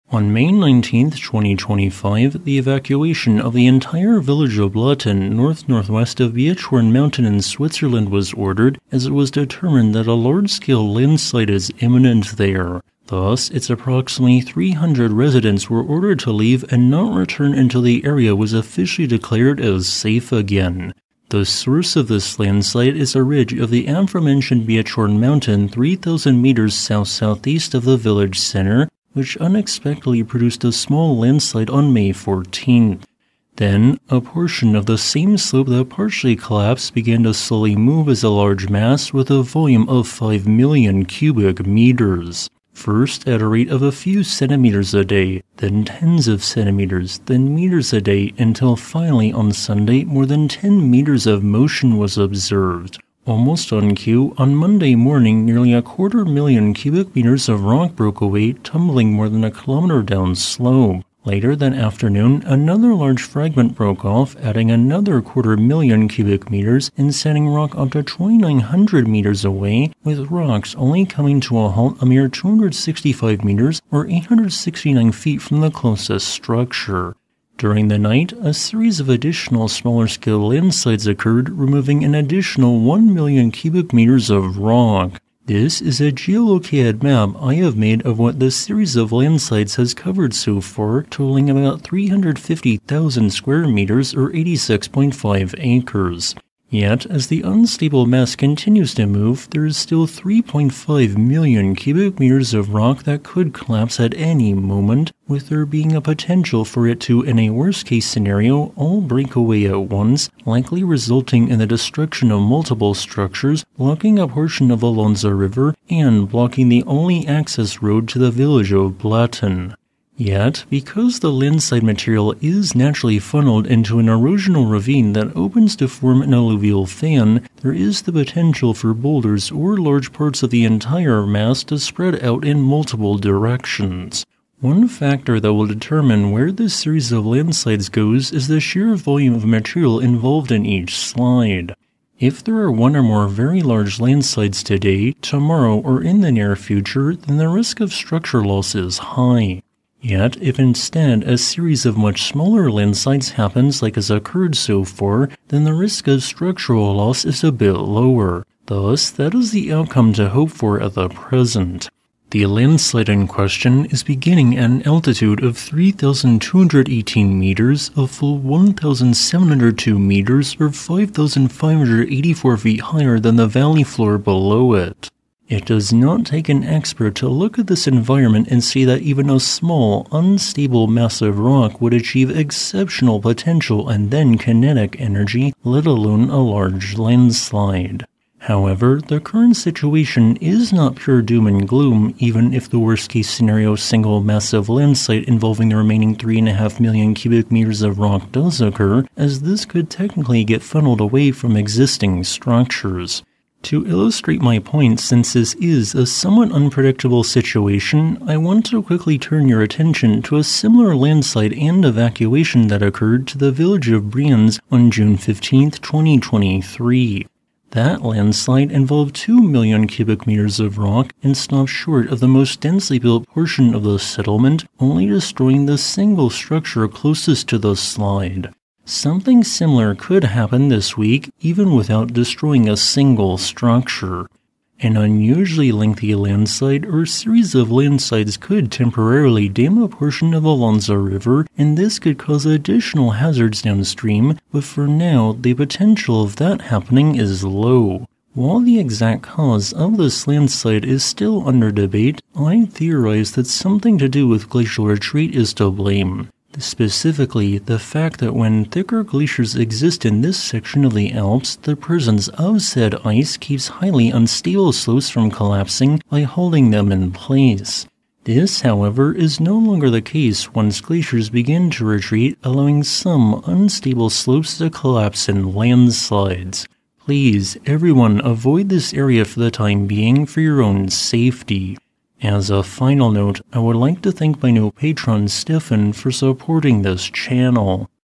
A Major Landslide is Imminent & Ongoing in Switzerland; Evacuations Ordered
A major landslide is imminent and ongoing in Switzerland which threatens a small town. Specifically, a landslide of 5 million cubic meters is expected to occur during the next 100 hours, placing Blatten village north of Bietschhorn mountain at risk. As a result, everyone who lives there has been ordered to evacuate. This will most likely occur either as a singular large slide or series of multiple smaller landslides. Today's video will discuss why this landslide is occurring and what might happen next.

Thumbnail Photo Credit: Dr. Richard Roscoe, Photovolcanica, Licensed Image

Sources/Citations:
[1] Mauro Häusler, Valentin Gischig, Reto Thöny, Franziska Glueer, Fäh Donat, Monitoring the changing seismic site response of a fast-moving rockslide (Brienz/Brinzauls, Switzerland), Geophysical Journal International, Volume 229, Issue 1, April 2022, Pages 299–310, CC BY 4.0. (This source describes more about the 2023 landslide elsewhere in Switzerland)

0:00 Imminent Landslide
0:29 Landslides so Far
1:37 Current Situation
3:23 Similar 2023 Landslide
4:08 Potential Landslide Cause